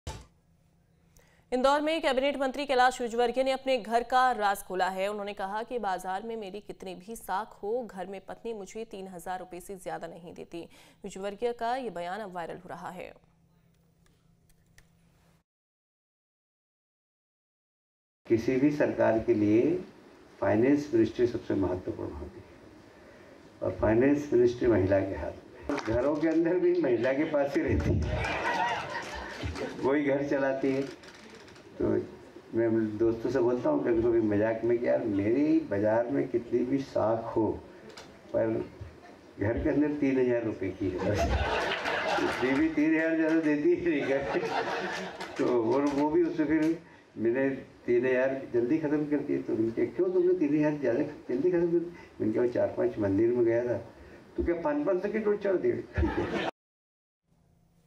Kailash Vijayvargiya ने खोला अपने घर का राज, कहा- पत्नी मुझे 3 हजार रुपये से ज्यादा नहीं देती
इंदौर में कैबिनेट मंत्री कैलाश विजयवर्गीय ने अपने घर का राज खोला है...उन्होंने कहा कि बाजार में मेरी कितनी भी साख हो घर में पत्नी मुझे 3 हजार रुपये से ज्यादा नहीं देती...विजयवर्गीय का यह बयान अब वायरल हो रहा है..

In Indore, Cabinet Minister Kailash Vijayvargiya has revealed the secret of his house... He said that no matter how much reputation I have in the market, my wife does not give me more than Rs 3 thousand at home... This statement of Vijayvargiya is now going viral. .